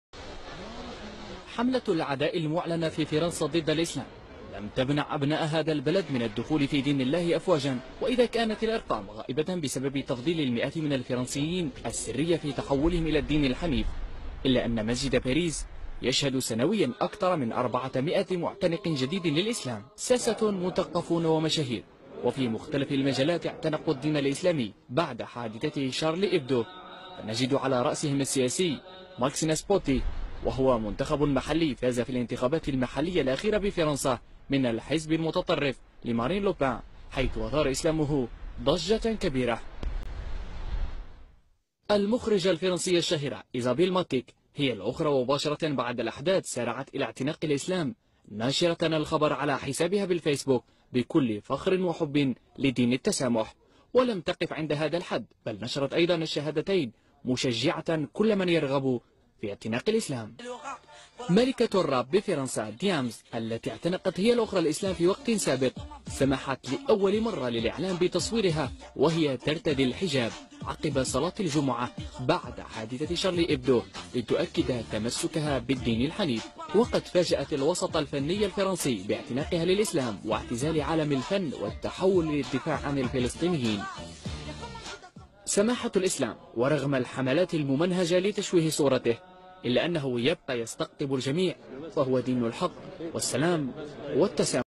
اسلام اشهر فنانة في فرنسا
تزايد عدد المسلمين في فرنسا